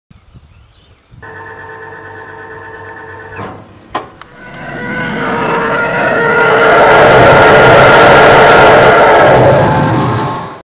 Star Wars à Montbazin
Star Wars à Montbazin hérault, des stormtroopers sur la place devant un AT-ST. 2 Tie Fighters passent au dessus du village devant des croiseurs impériaux